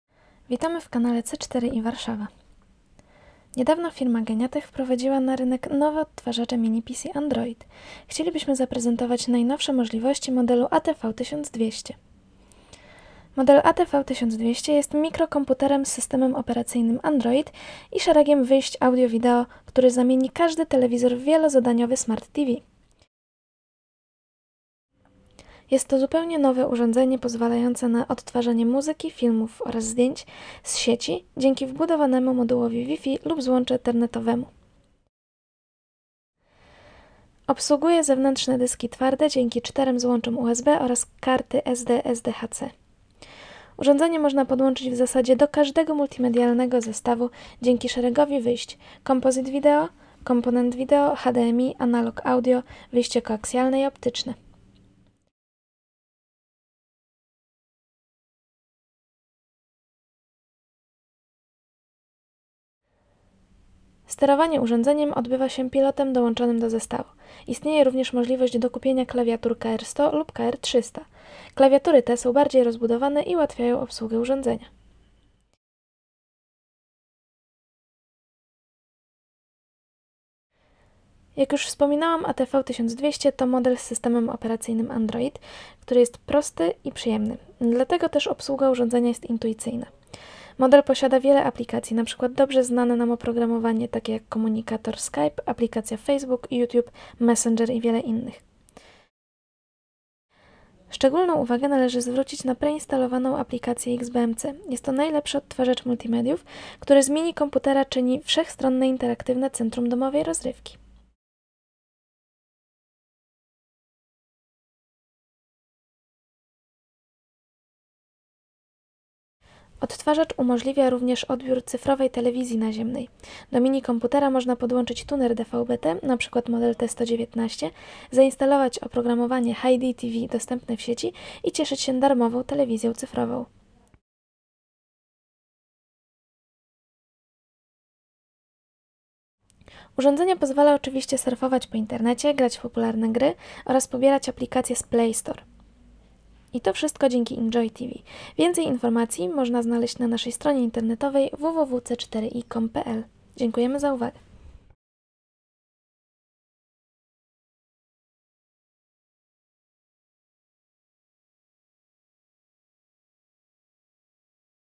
Mini Android PC ATV1200 NOWOŚĆ
Kliknij w "Pokaż więcej" aby rozwinąć

Film przedstawiający funkcjonalność oraz specyfikację techniczną najnowszego odtwarzacza multimedialnego firmy Geniatech.

Masz pytania?

Doradzimy najlepsze rozwiązanie dla Ciebie!

Zapraszamy!